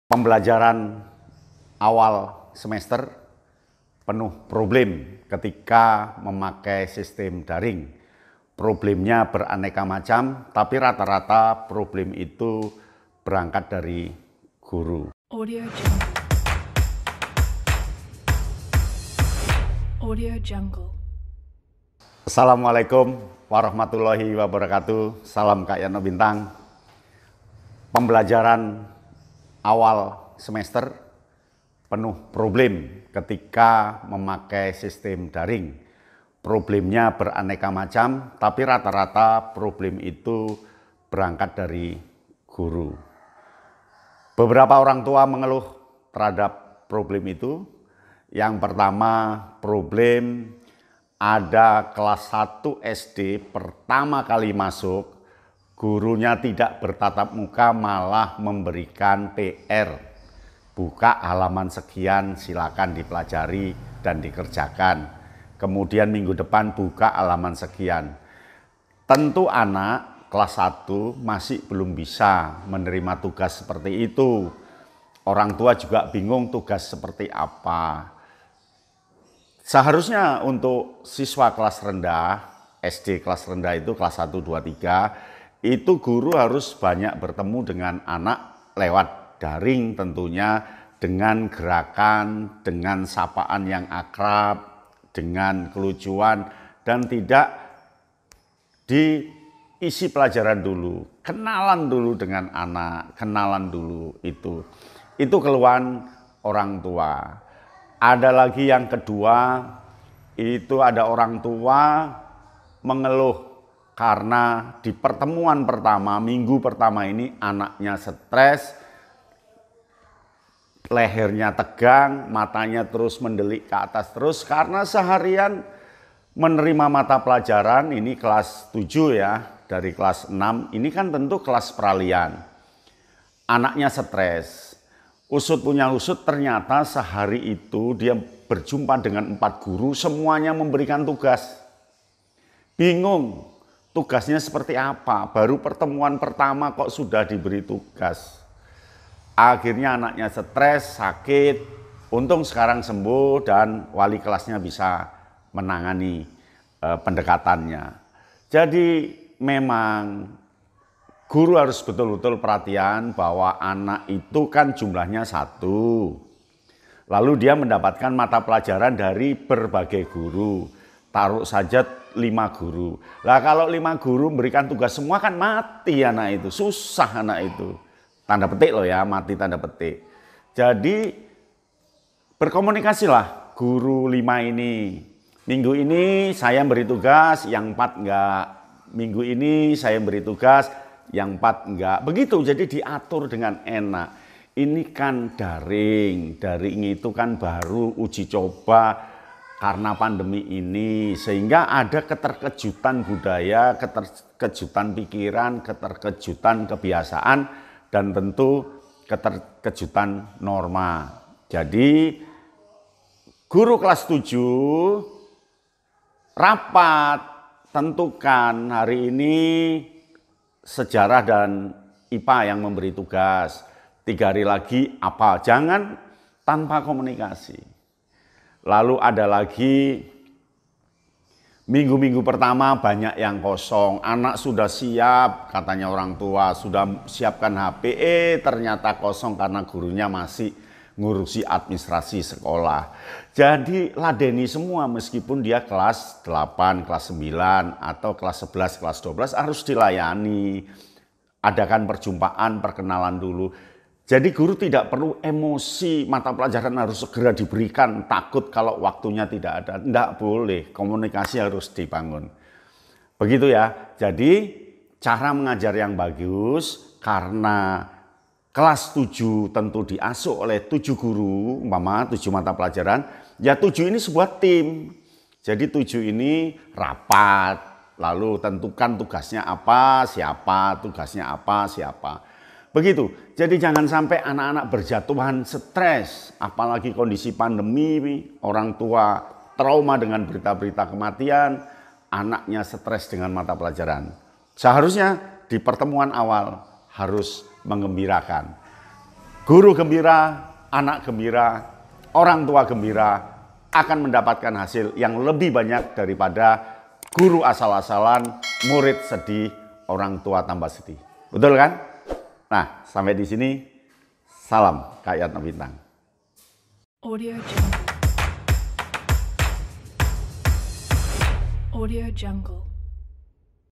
Problem Guru dalam Pembelajaran Daring
Saat daring, tidak semua guru dapat melaksanakan pembelajaran dengan tepat  Problem guru dalam pembelajaran daring. Cara sukses belajar daring. Langkah pembelajaran daring.